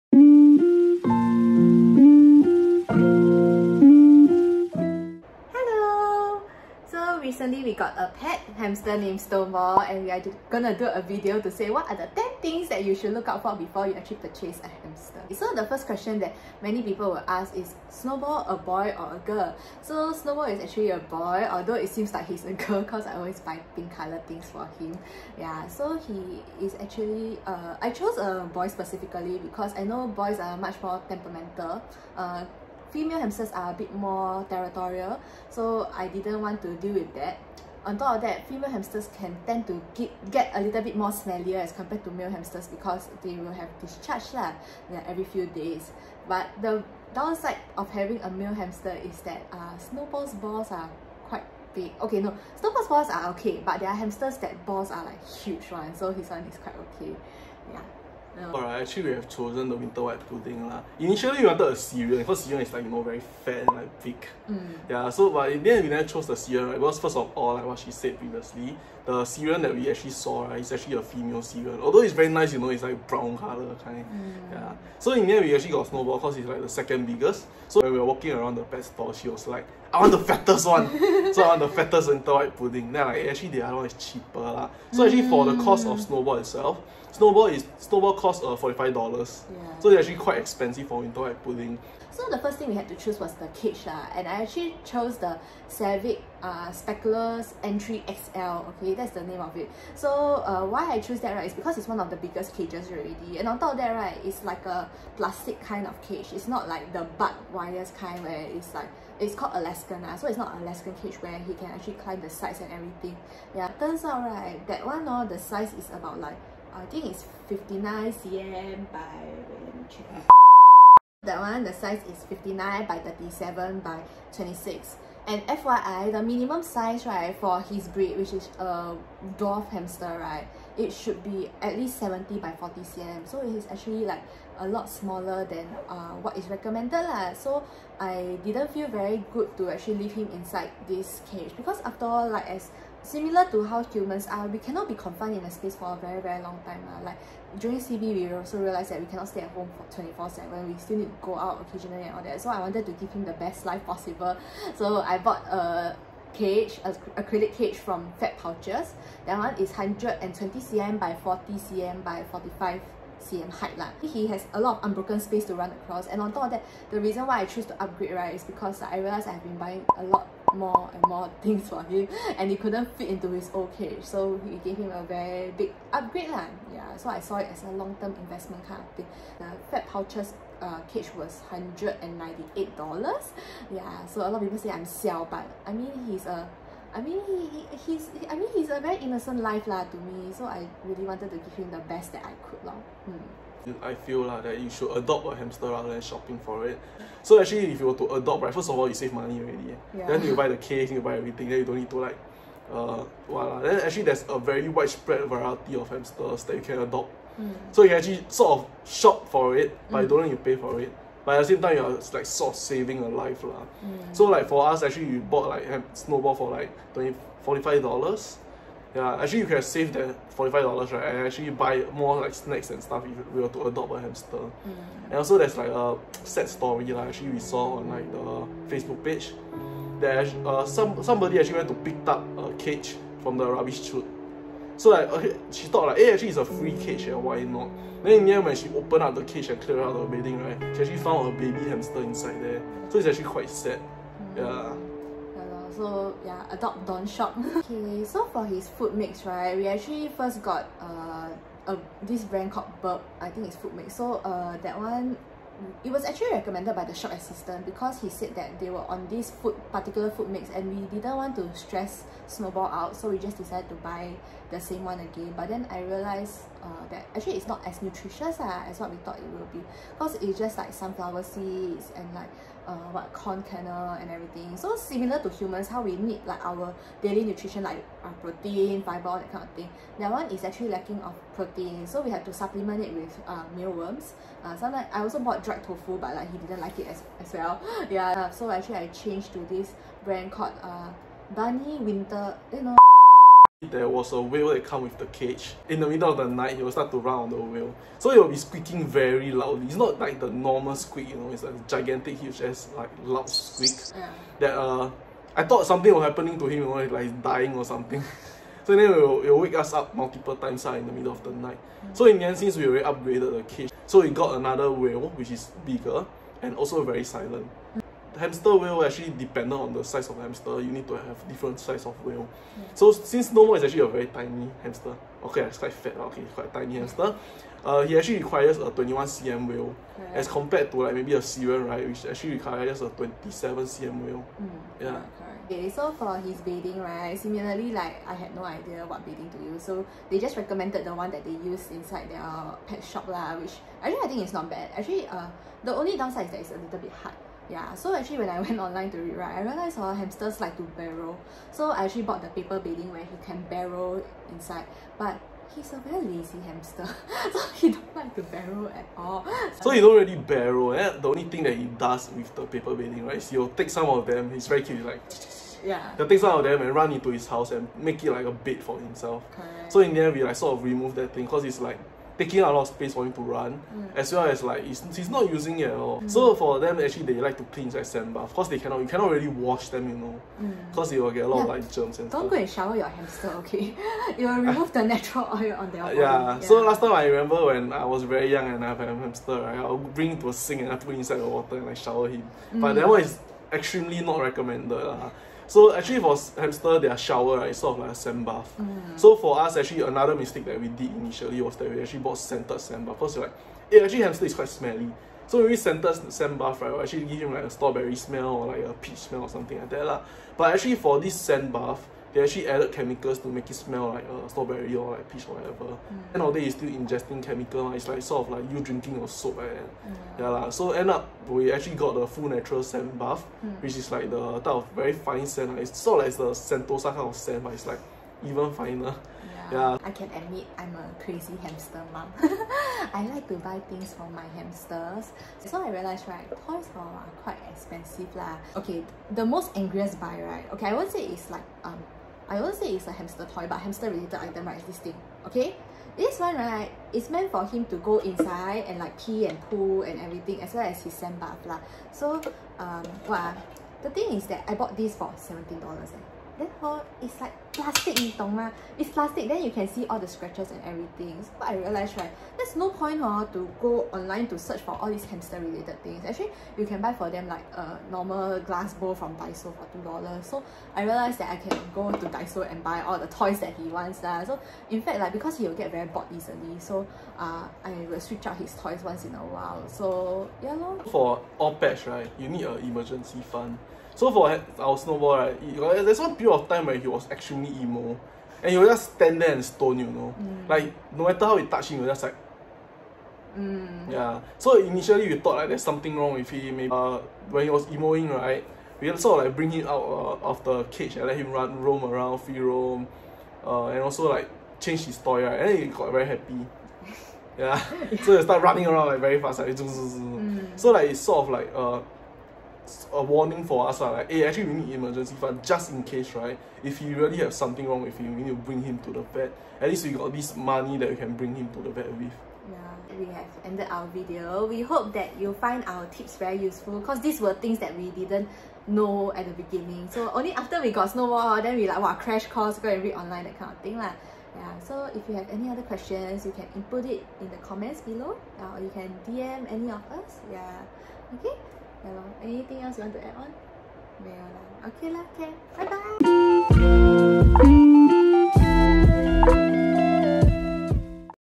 10 THINGS TO KNOW BEFORE GETTING A HAMSTER 🐹
I used to be a clueless hamster mom but look at me now!! Snowball has bought great joy to my life and I hope that if you wanna get a furball you would not make the same mistakes as I did.